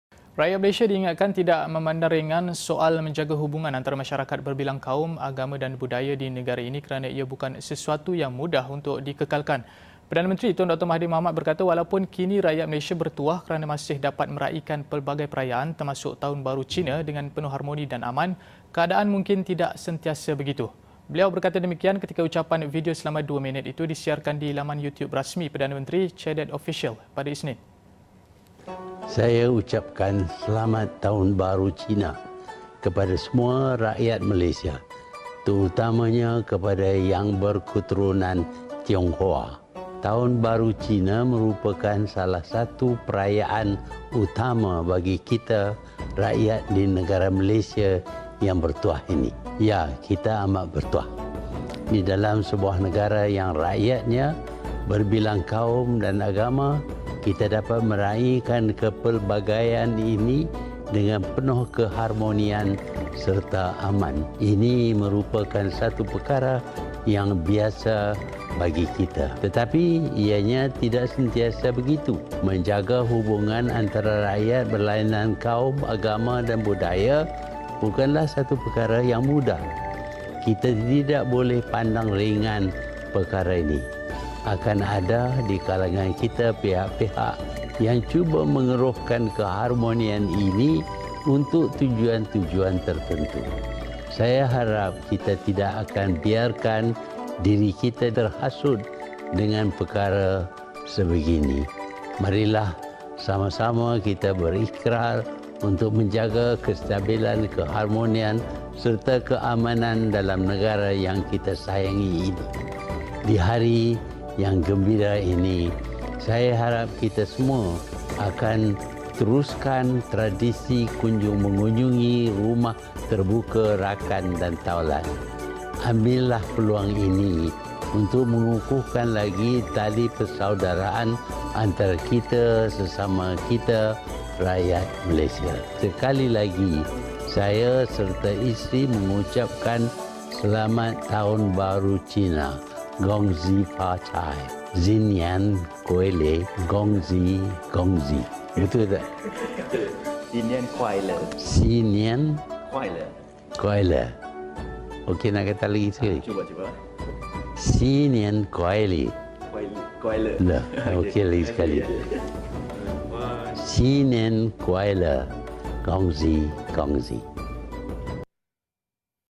Utamakan kepelbagaian kaum, jangan keruhkan keharmonian
Rakyat Malaysia diingatkan tidak memandang ringan soal menjaga hubungan antara masyarakat berbilang kaum, agama dan budaya di negara ini kerana ia bukan sesuatu yang mudah untuk dikekalkan.